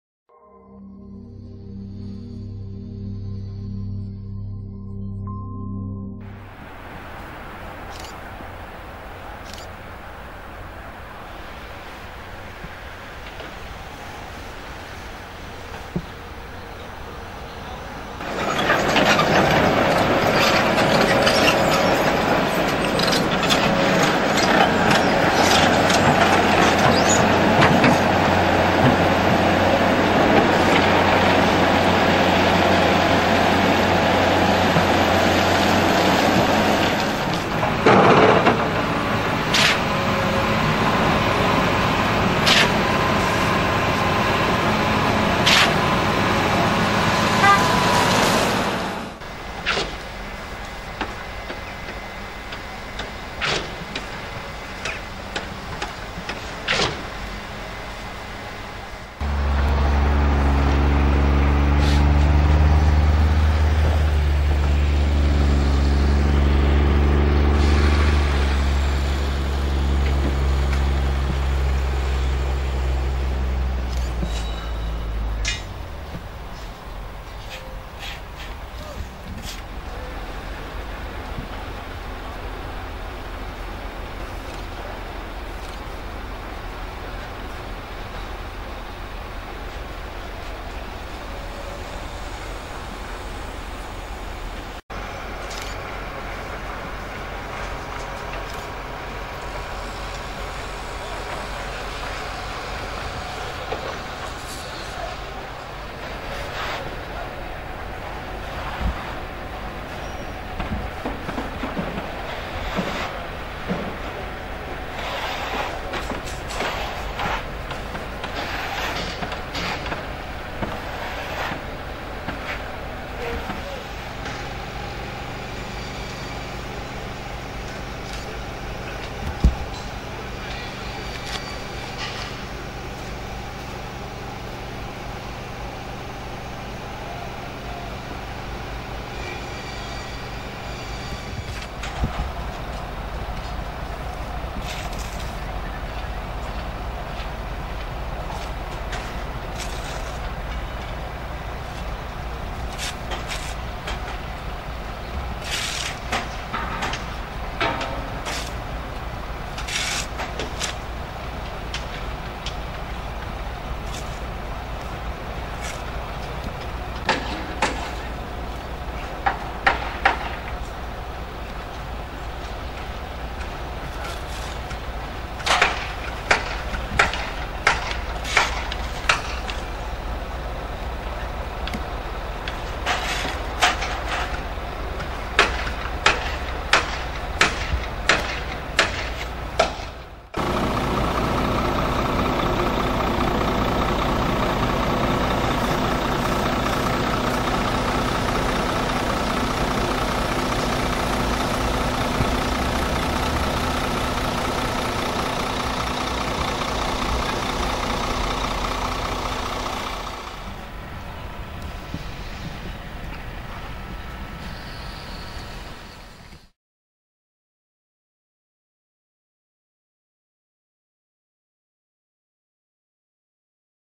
Budowa stadionu w Tychach - stan inwestycji na dzień 31 października 2013 r.
Materiał video obrazujący postęp prac prowadzonych na terenie budowy Stadionu Miejskiego w Tychach na dzień 31 października 2013 r.